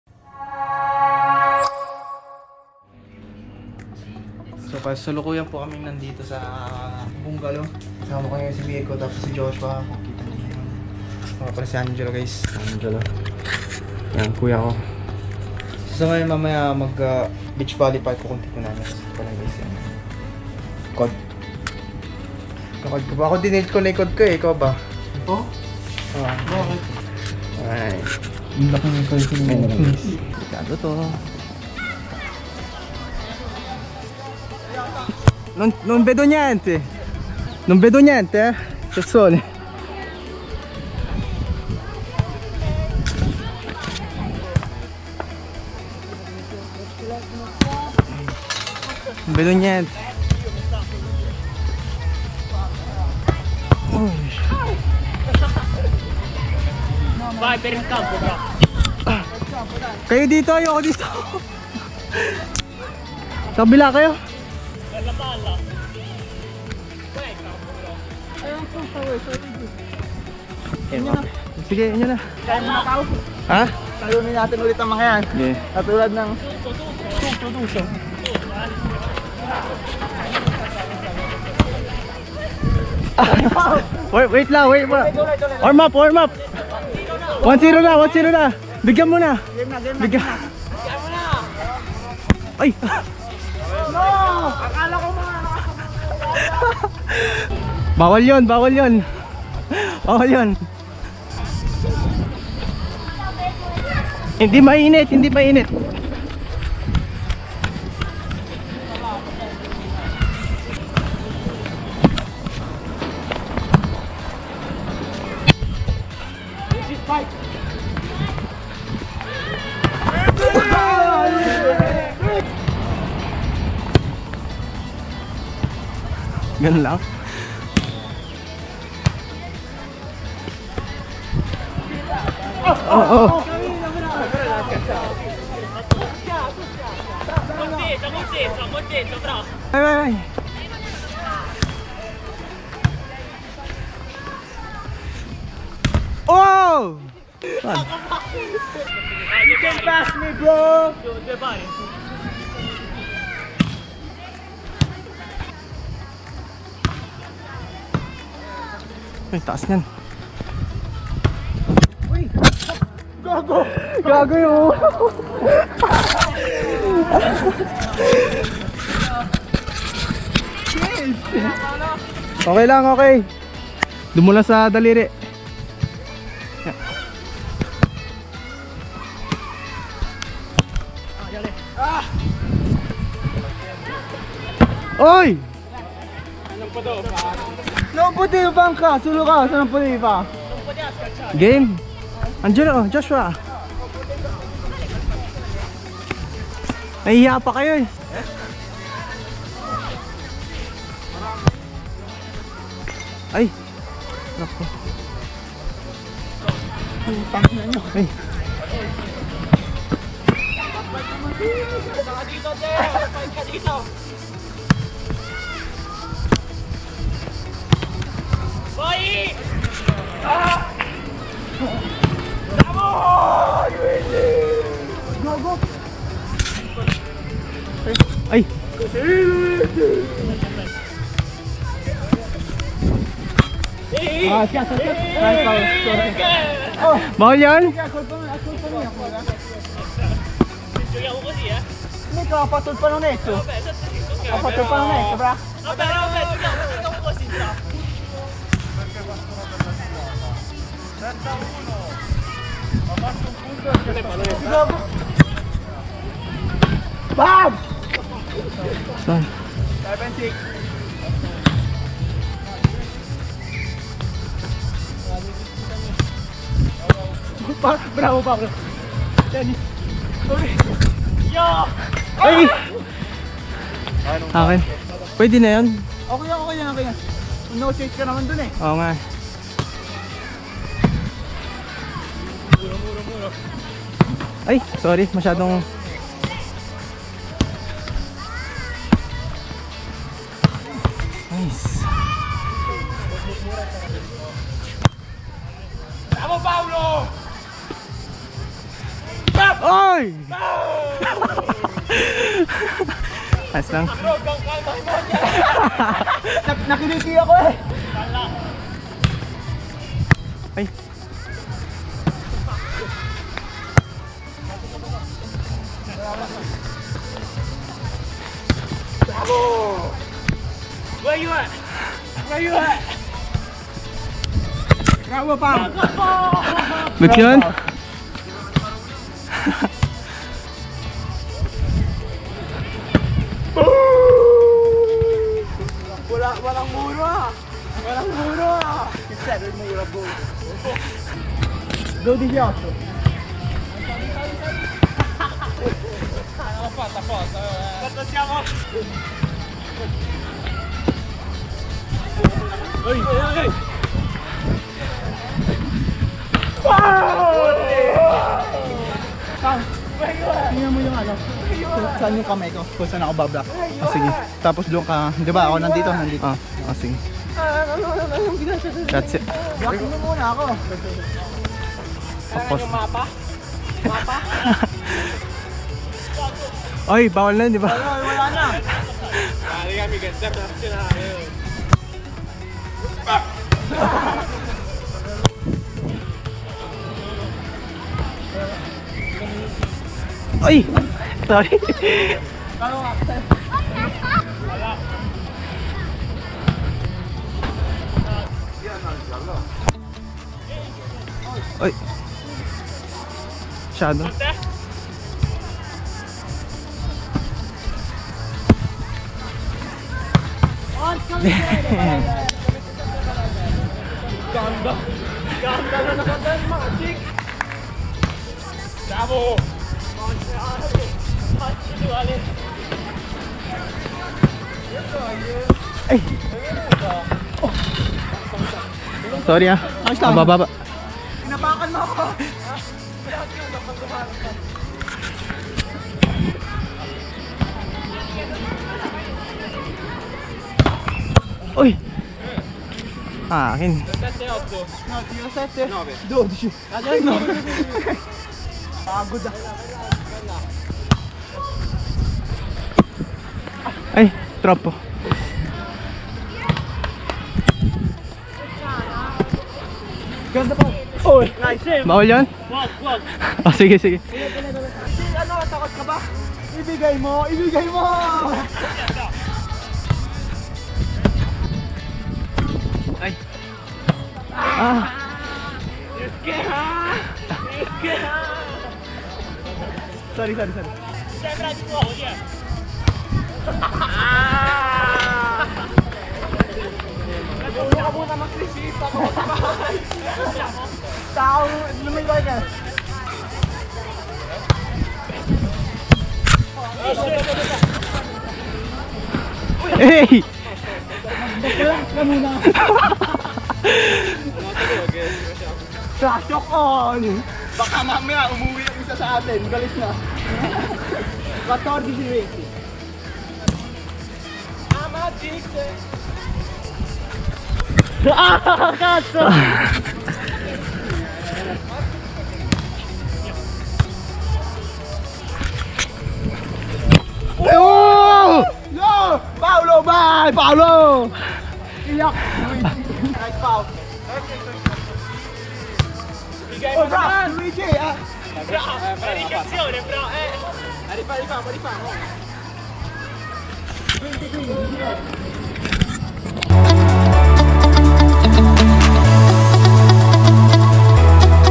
PauHD Films | FABULOUS VILLAGE BEACH VOLLEY 2021 // MATCH 1🤣 - #80 GoPro
Description :

AFTER A YEAR WE ARE BACK AGAIN 2021
Fabulous Village Beach Volley 2021 - GoPro Hero 7 Black // MATCH 1, SET 1
DAY 1